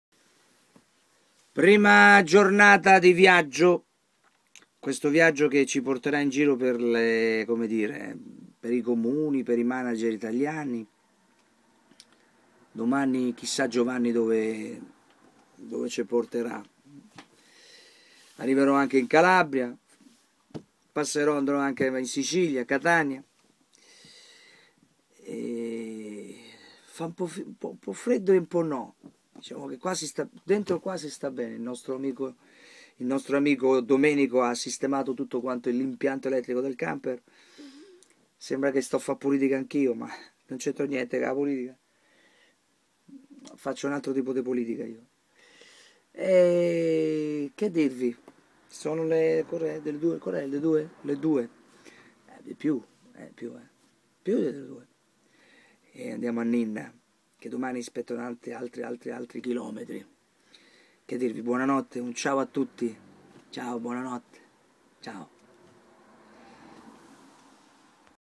Il viaggio
Andiamo a trovare i nostri amici manager per organizzare il tour estivo.